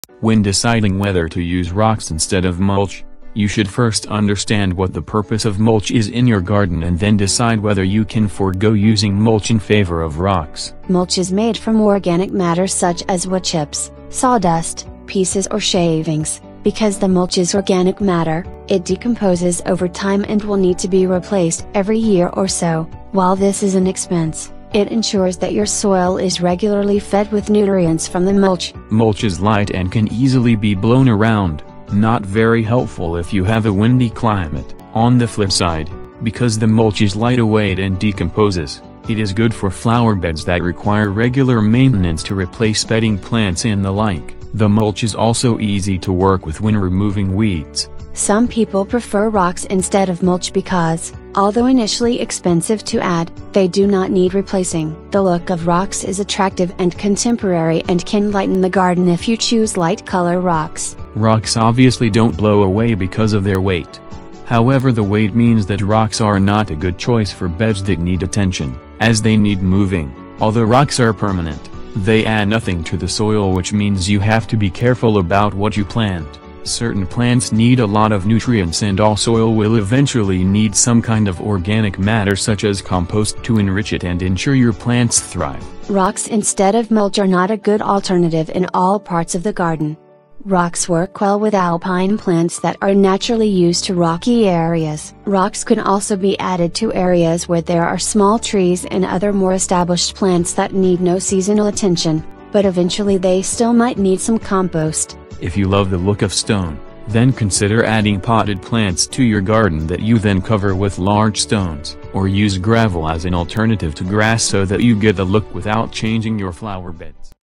Should I Use Rocks Instead of Mulch?
When deciding whether to use rocks instead of mulch, you should first understand what the purpose of mulch is in your garden and then decide whether you can forego using mulch in favor of rocks.

Mulch is made from organic matter such as wood chips, sawdust, pieces or shavings. Because the mulch is organic matter, it decomposes over time and will need to be replaced every year or so. While this is an expense, it ensures that your soil is regularly fed with nutrients from the mulch.

Mulch is light and can easily be blown around, not very helpful if you have a windy climate. On the flipside, because the mulch is lightweight and decomposes, it is good for flowerbeds that require regular maintenance to replace bedding plants and the like. The mulch is also easy to work with when removing weeds.

Some people prefer rocks instead of mulch because, although initially expensive to add, they do not need replacing. The look of rocks is attractive and contemporary and can lighten the garden if you choose light color rocks.

Rocks obviously don’t blow away because of their weight. However the weight means that rocks are not a good choice for beds that need attention, as they need moving. Although rocks are permanent, they add nothing to the soil which means you have to be careful about what you plant. Certain plants need a lot of nutrients and all soil will eventually need some kind of organic matter such as compost to enrich it and ensure your plants thrive.

Rocks instead of mulch are not a good alternative in all parts of the garden. Rocks work well with alpine plants that are naturally used to rocky areas. Rocks can also be added to areas where there are small trees and other more established plants that need no seasonal attention, but eventually they still might need some compost.

If you love the look of stone, then consider adding potted plants to your garden that you then cover with large stones. Or use gravel as an alternative to grass so that you get the look without changing your flowerbeds.